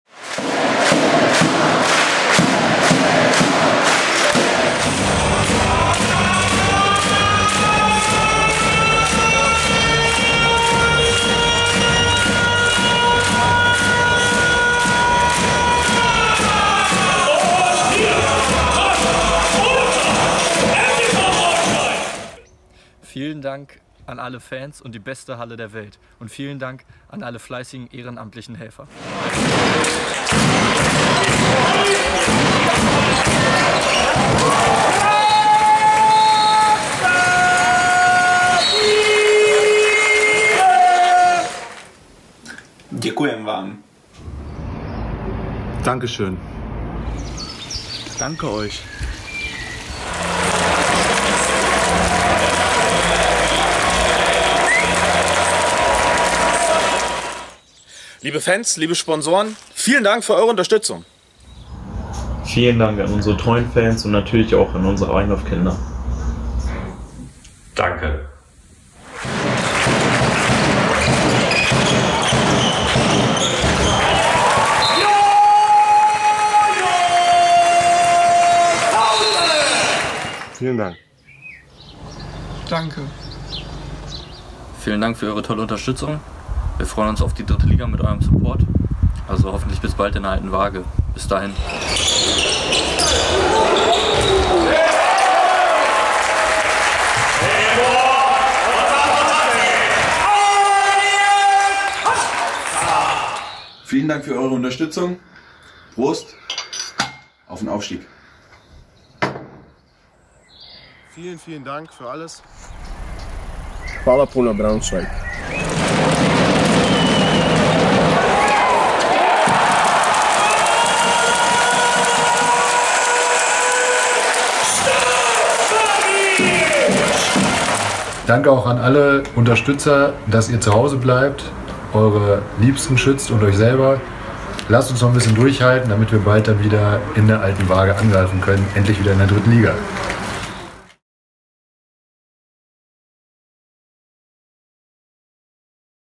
Der MTV sagt Danke
Die Saison des MTV Braunschweig fand, wegen des Coronavirus, leider vorzeitig ihr Ende. Dennoch wollen wir Danke sagen. Vielen Dank für eure Unterstützung.
Wir freuen uns darauf, in der kommenden Saison wieder in der 3. Liga zu spielen und wir freuen uns, euch wieder in unserer Alten Waage begrüßen zu dürfen.